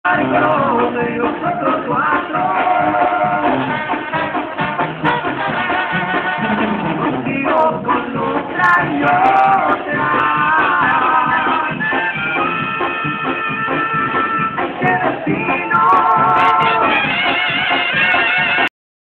caballos espanoles el espejo y el cuervo
caballos bailando en  fiesta privada